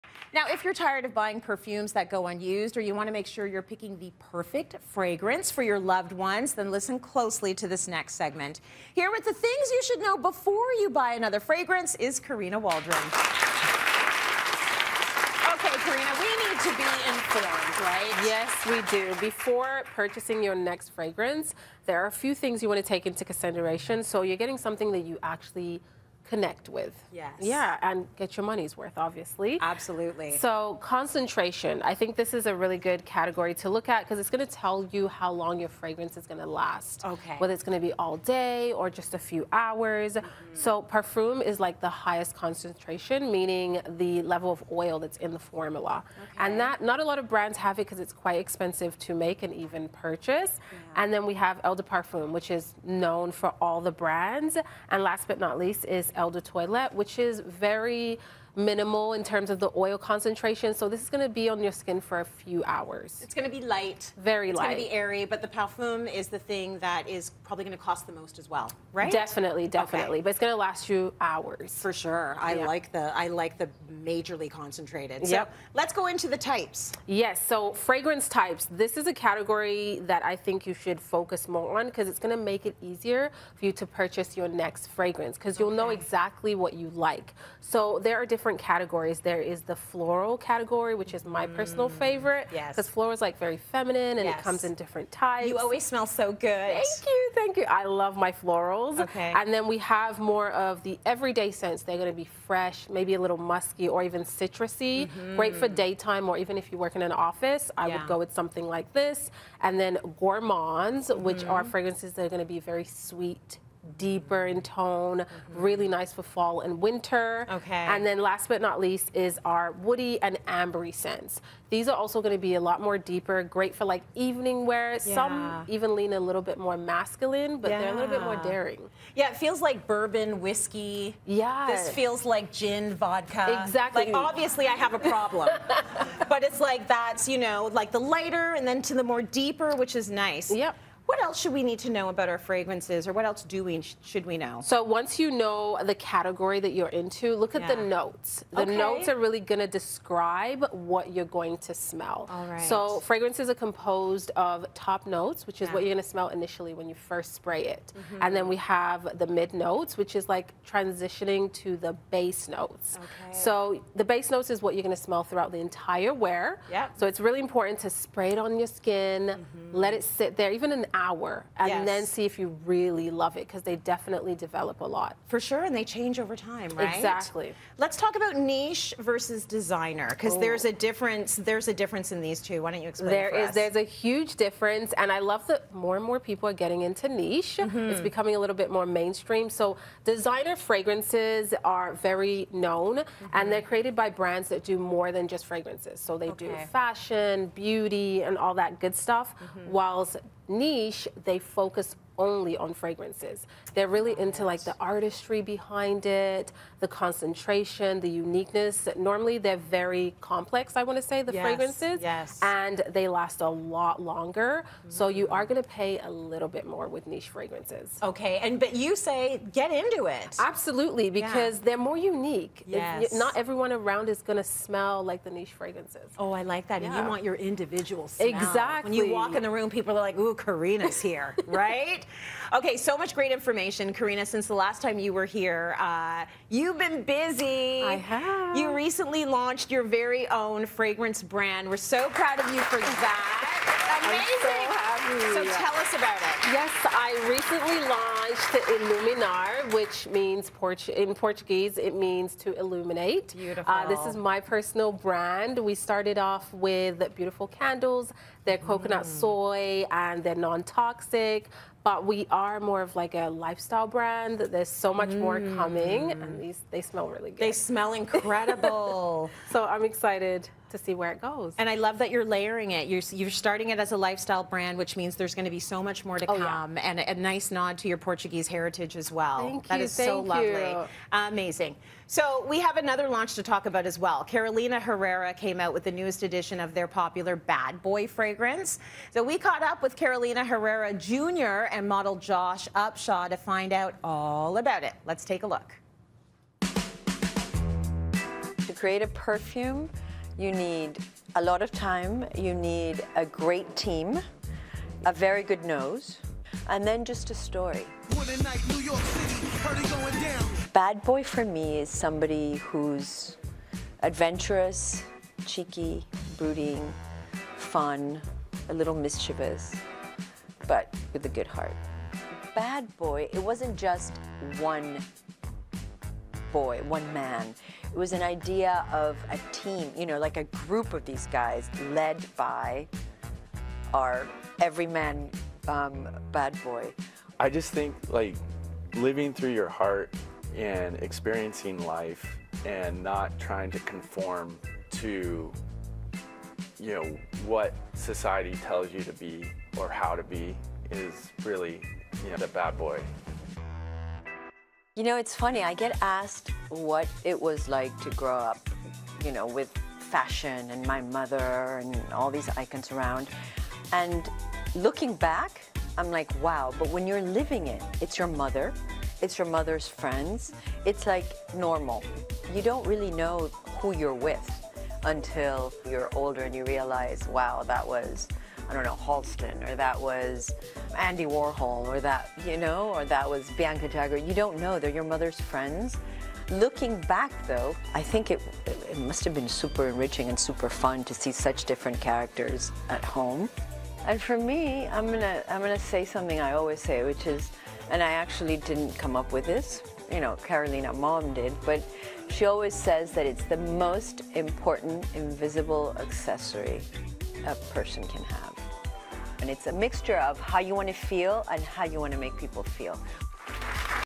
The 4 things you should know before buying a fragrance
Fragrance expert Karina Waldron is here with ALL the things you should know before you buy any fragrance — and Carolina Herrera Jr. and model Josh Upshaw talk about the newest edition of the popular Bad Boy fragrance.